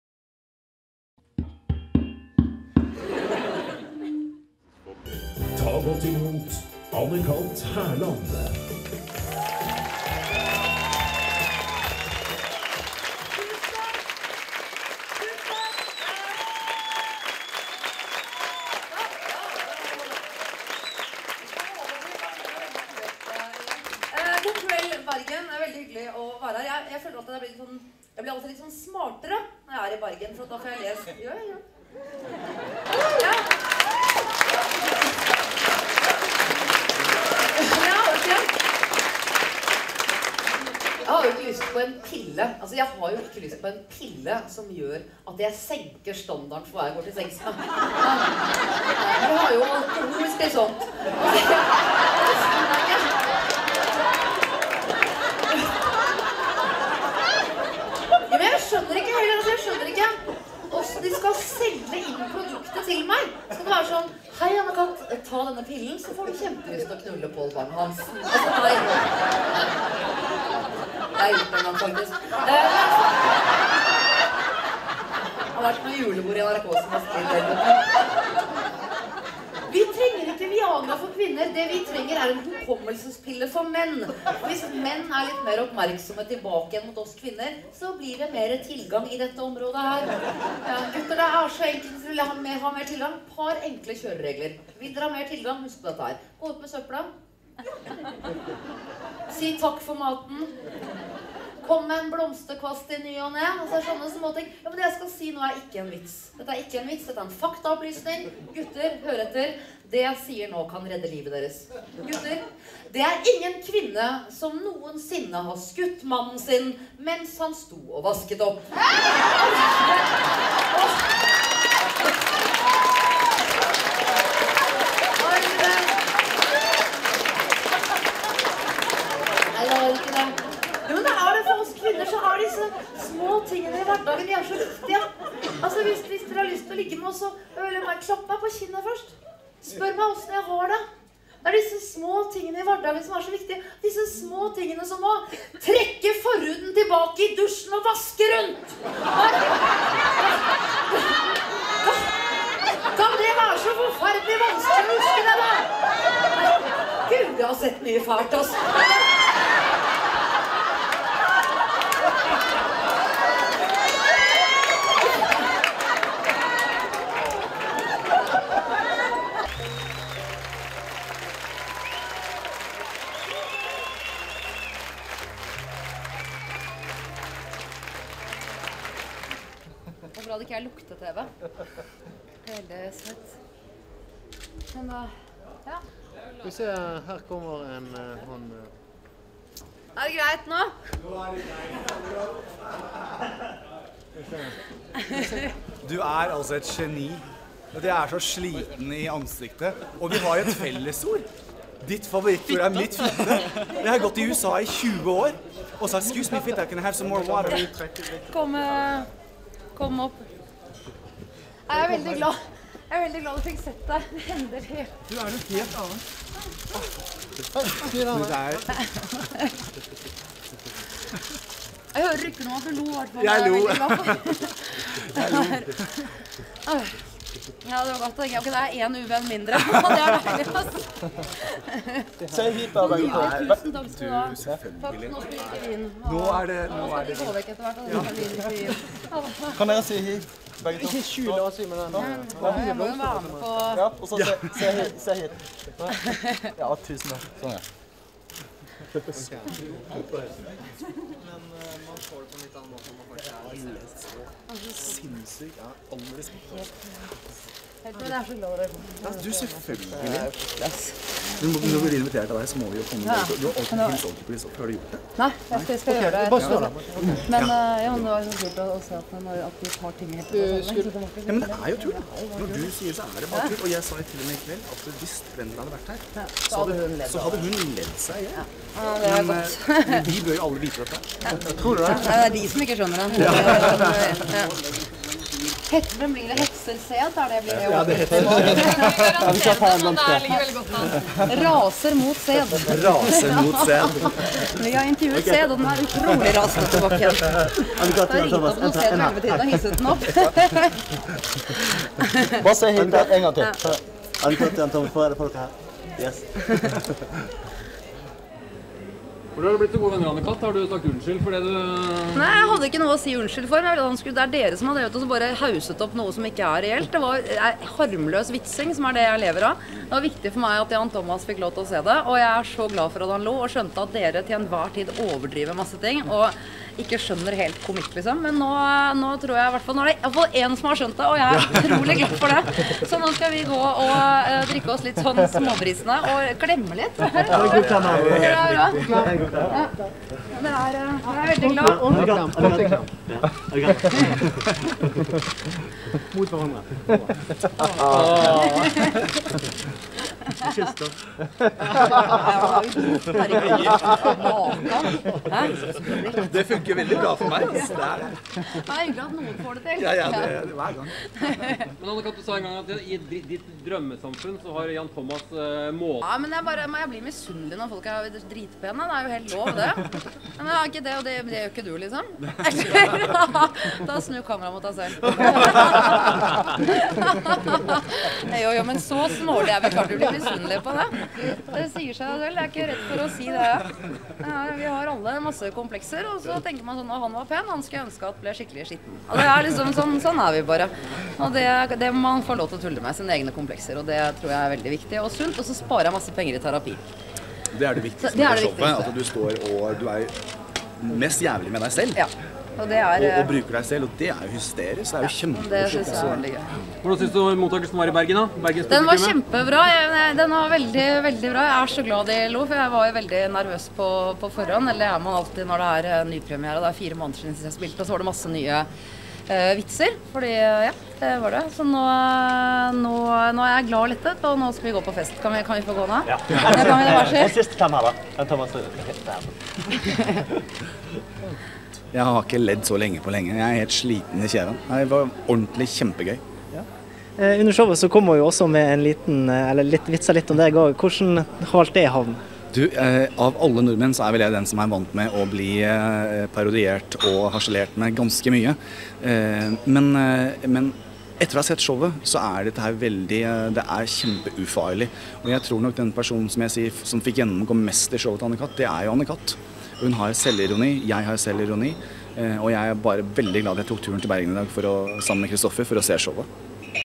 Anne Kat Hærland - Årets komiker 2010
Anne Kat Hærland ble årets komiker i 2010. Her er et utdrag av showet hennes i Bergen høsten 2009. Og sjekk pressekonferansen etterpå.

Sjekk for mer info om komikerene.